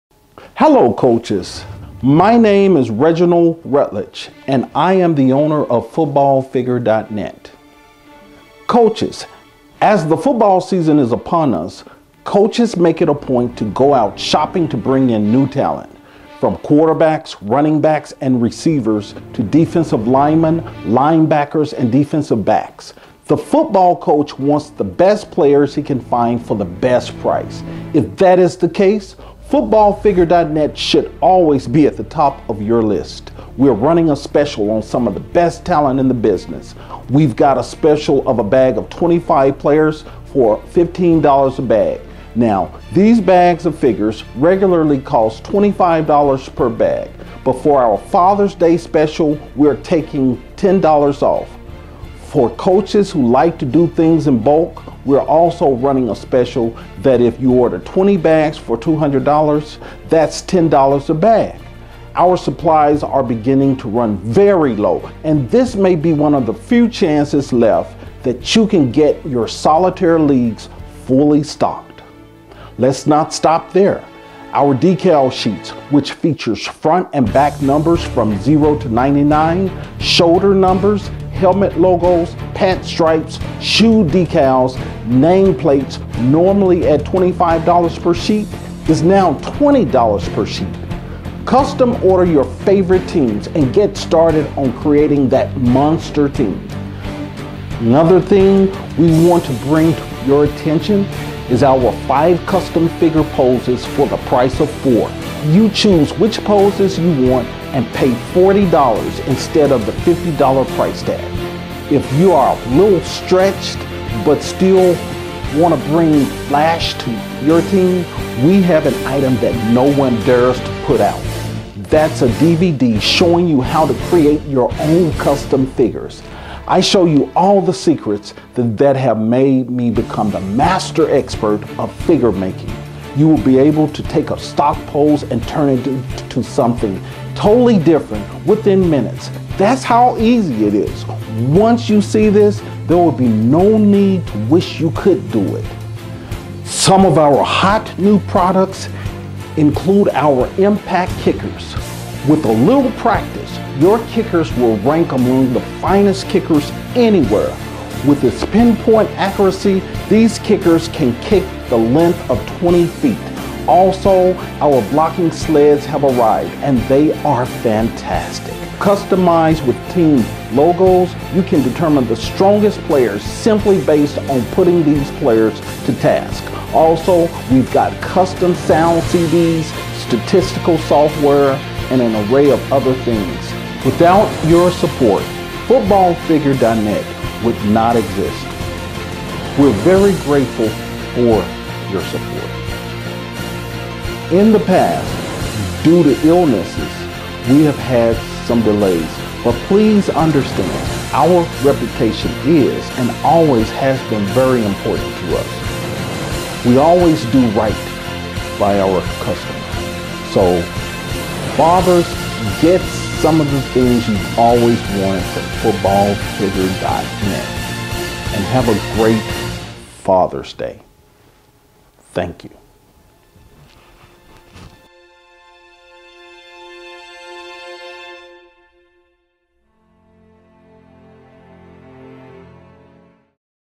Footballfigure.net Father's Day Sale
A 3-day miniature football sale with incredible prices for the
miniature football coach